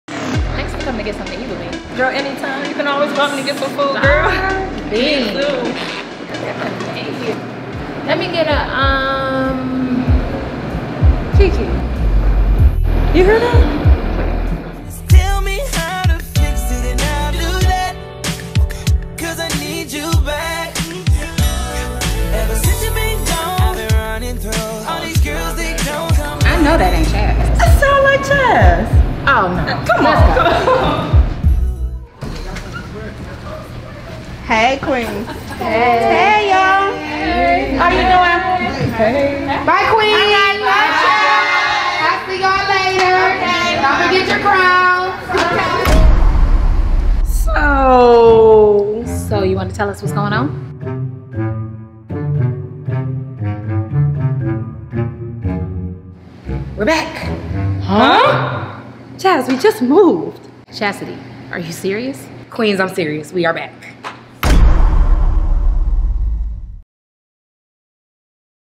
The Return of The Queens
Myself  and my two instructors made a temporary move of our dance classes for two months at the top of 2018... It was a much smaller space and we were unhappy... So we did this promo video with Drico Lamar to announce our return!